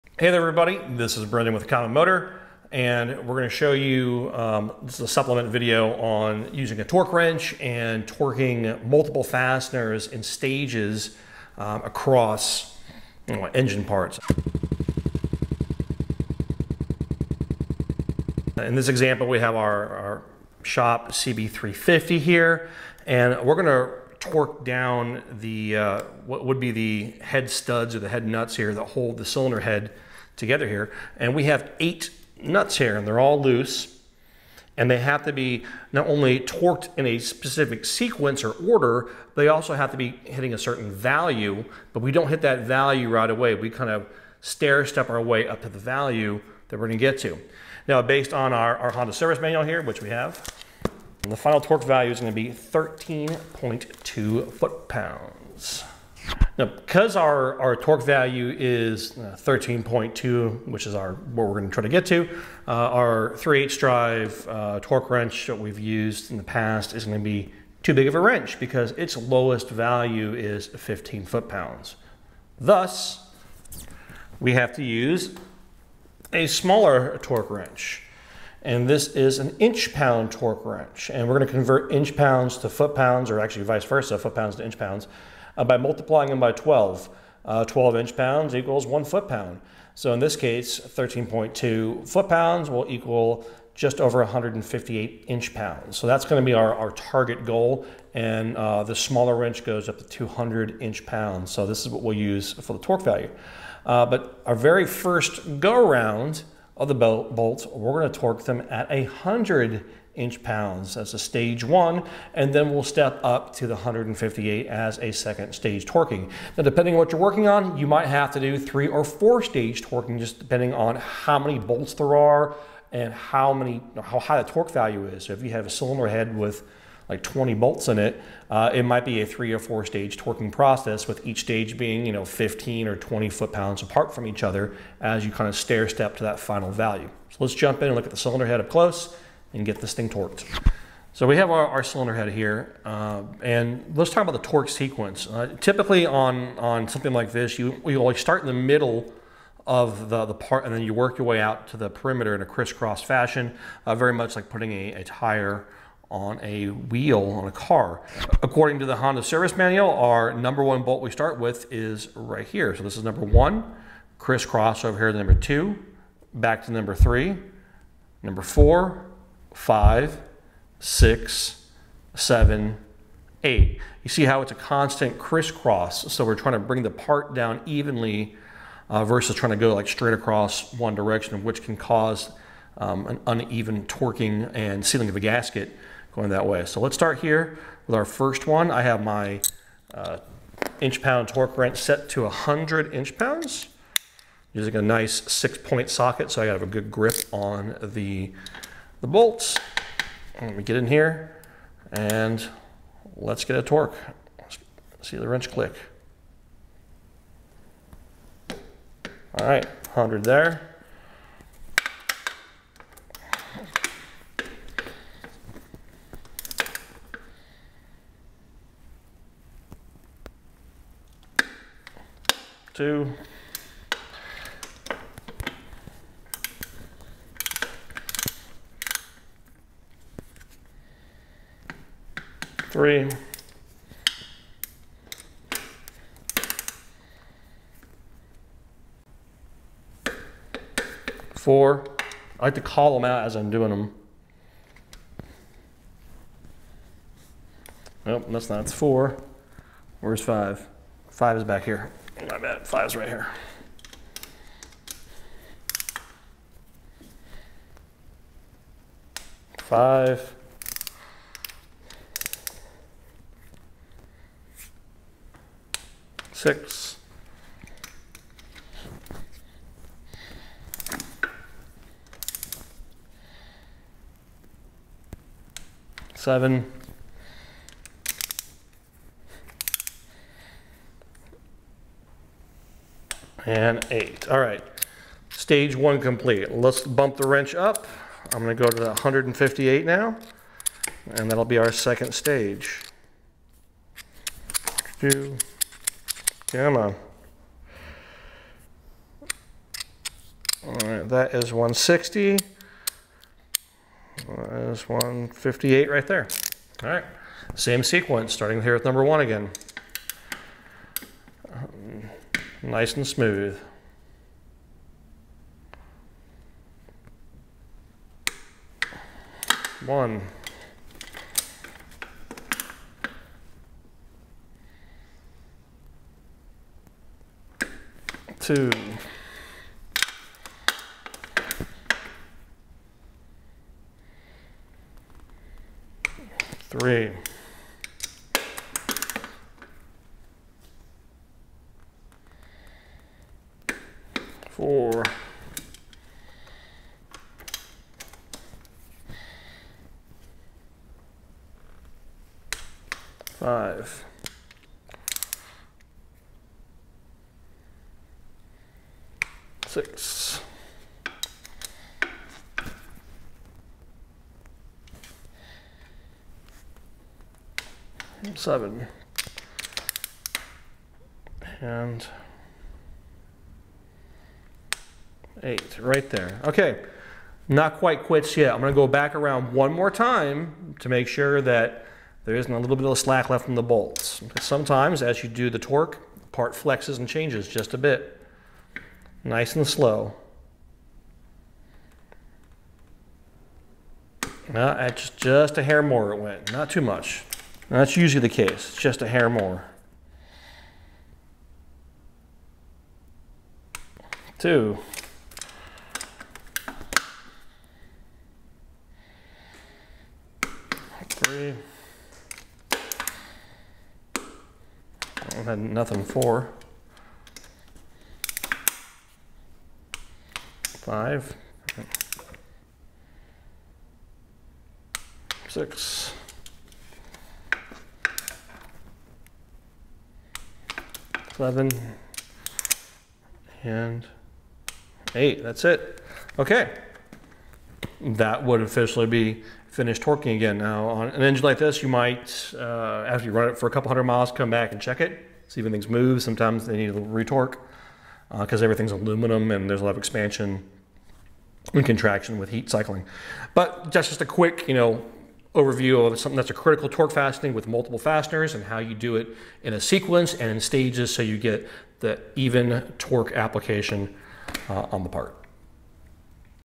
How to Torque Bolts in Sequence on your Vintage Honda Motorcycle
Torquing bolts in a sequence is key for critical parts like engine side covers and head bolts on your vintage motorcycle. Learn how to properly follow the manual torque sequence and our tips and tricks for success along the way. If you are new to using a torque wrench, check out our how to use a torque wrench video right here:

Contents of this video:

00:00 - Intro
00:19 - What application would I do this in?
02:21 - The torquing process
07:33 - Summary